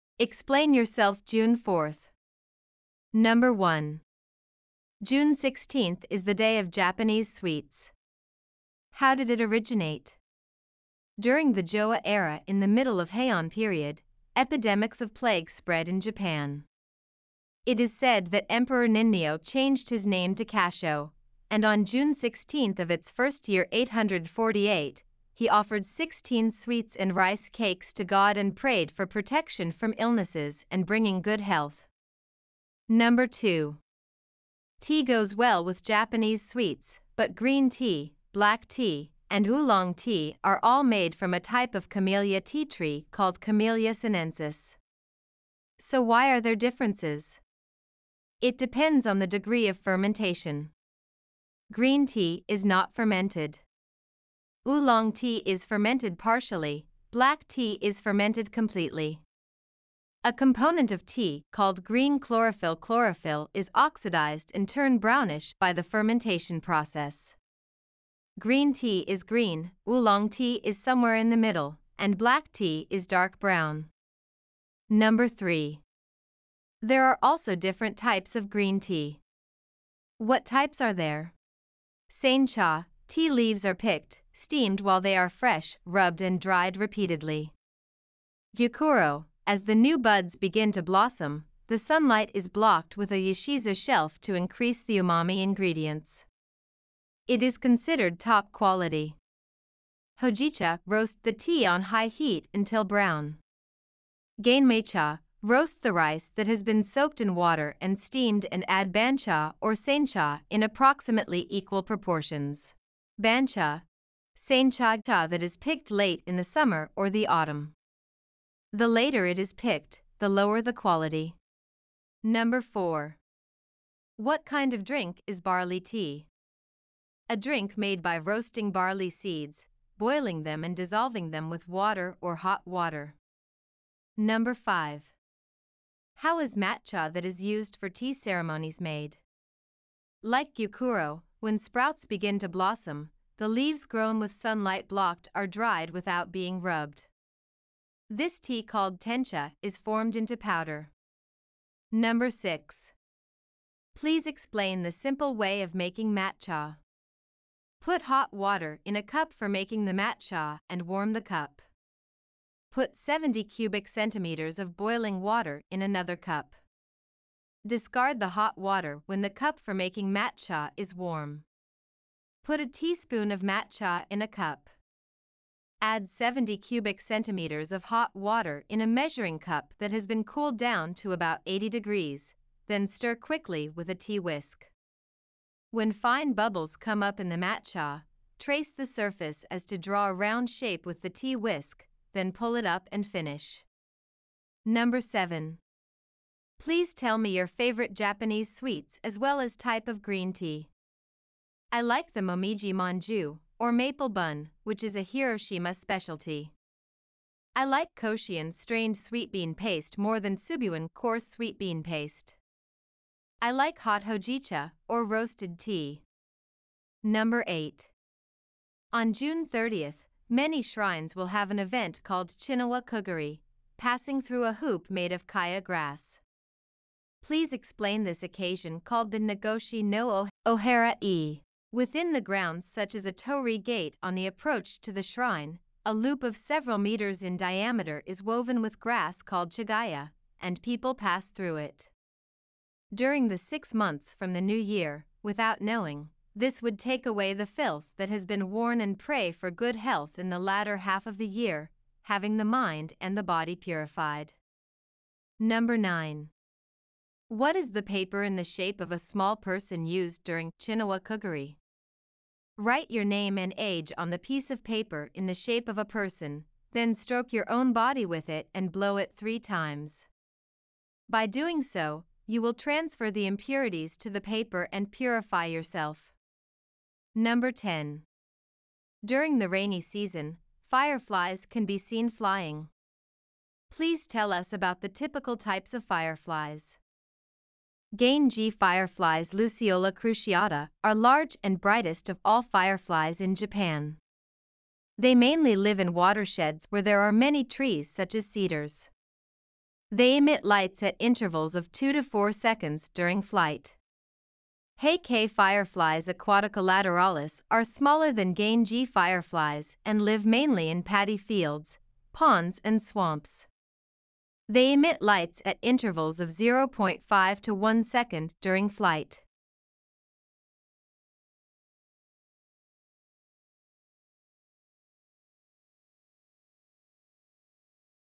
Explain Yourself June 4
Explain Yourself ！
あなたの言葉を使って　日本のことを外国人に説明できるようになりましょう
これは日本のことをしっかり英語で説明する練習をするための動画です。
Readingm 教材として使っていただいてもいいし　Listening 教材として使っていただいてもいいです。
モデルアンサーはあくまで参考程度にしていただいて、自分の英語で説明してみる練習をされることをお勧めいたします。
日本のことを英語で説明することを通じ、日本の伝統、日本の良さをあらためて感じていただければ幸いです。

2025年、大阪万博が開かれます。世界中の人を歓迎できる自分になるために楽しく英語を勉強しましょう！

No 1 0:03 和菓子
N0.2 0:32. お茶の種類
No.3 1:14  緑茶の種類
No.4  2:01　麦茶
No.5  2:14　抹茶の作り方
No.6  2:31 　抹茶のたて方
No.7   3:14　あなたの好きな和菓子と抹茶
No.8   3:37　茅の輪くぐり
No. 9  4:15　ひとかたとは？
No.10  4:37　蛍